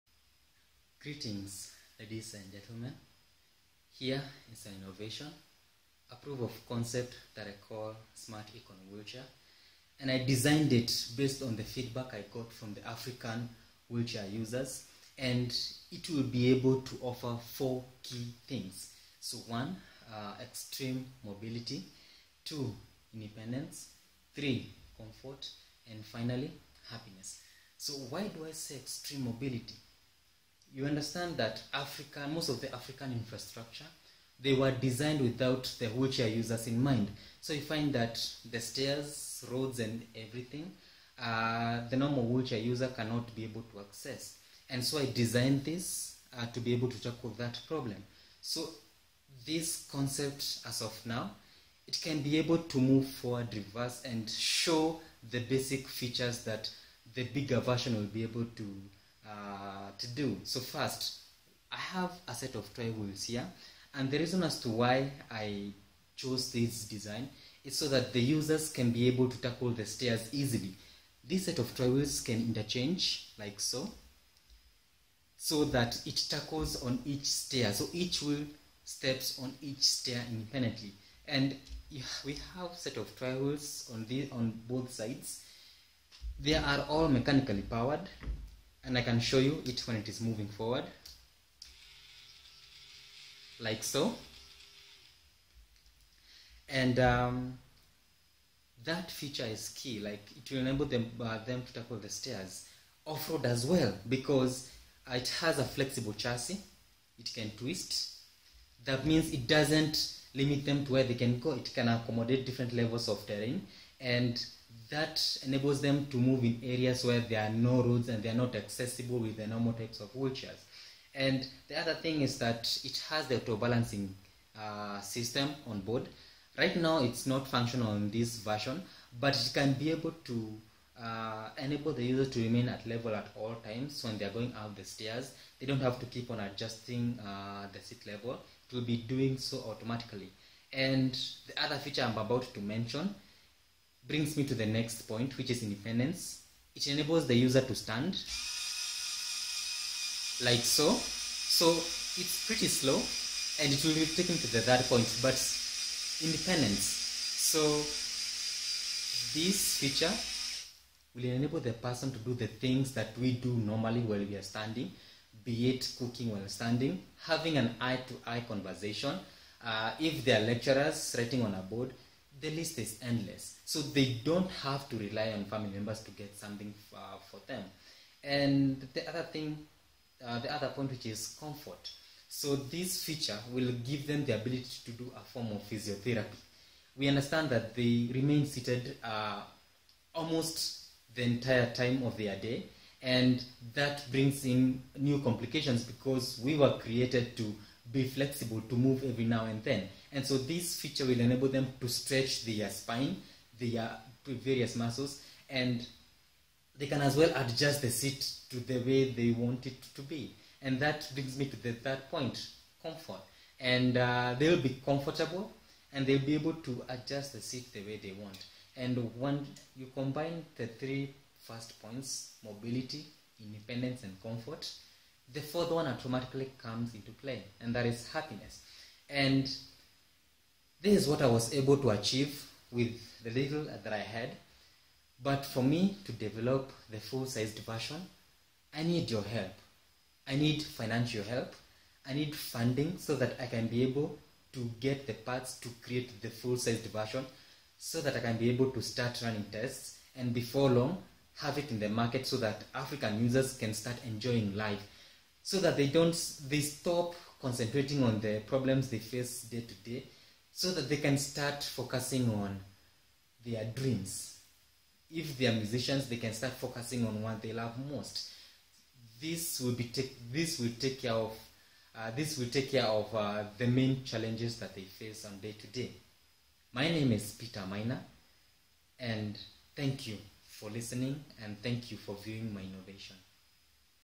SMart E Con Wheelchair
This is a Smart E-Con wheelchair concept that I designed based on the feedback I got from local wheelchair users. Most African Infrastures were not designed with wheelchair users in mind. It will offer Extreme mobility, independence, comfort and happiness to disabled persons